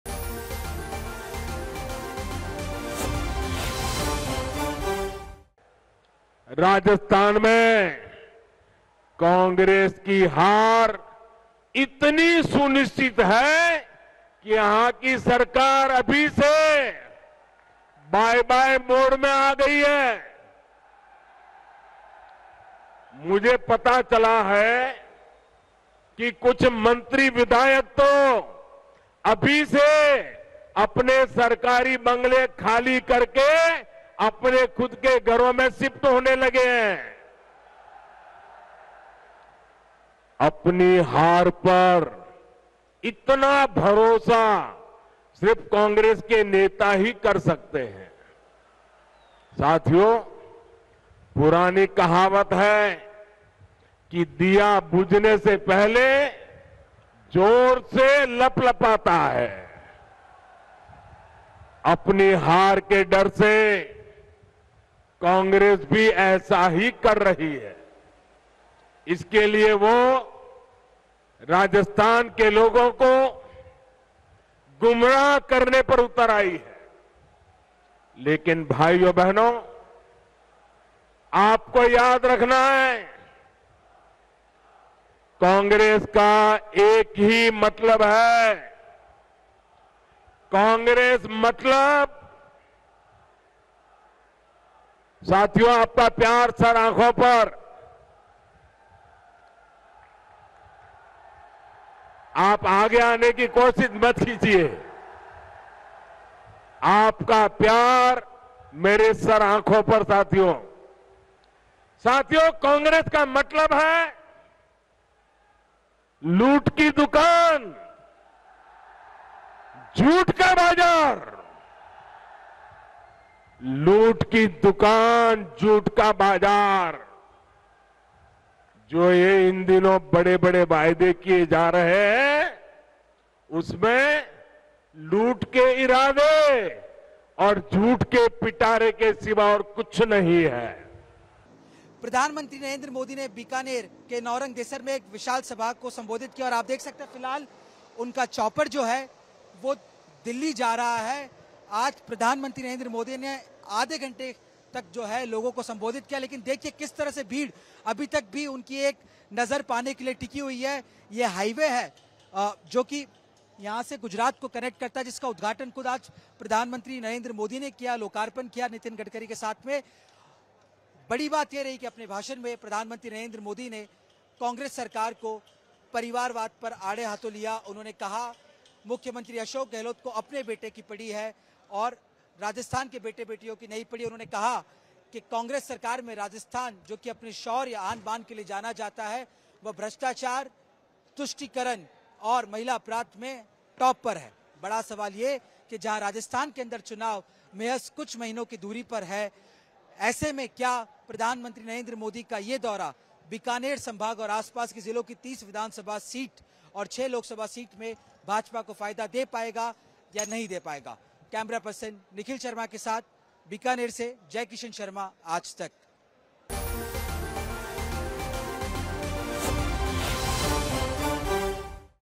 Rajasthan Assembly Polls: PM Narendra Modi ने बीकानेर के नौरंगदेसर में साधा गहलोत सरकार पर निशाना
प्रधानमंत्री नरेंद्र मोदी ने बीकानेर के नौरंगदेसर में एक जनसभा को संबोधित करते हुए राज्य की अशोक गहलोत सरकार पर ज़ोरदार निशाना साथा । उन्होंने कहा कि कांग्रेस सरकार ने 4 सालों में राजस्थान का बहुत नुकसान किया है और ये बात कांग्रेस के नेता भी अच्छी तरह जानते हैं। राजस्थान में कांग्रेस की हार इतनी सुनिश्चित है कि यहां की सरकार बॉय-बॉय मोड में आ गई है, कांग्रेस का मतलब है- लूट की दुकान, झूठ का बाजार ।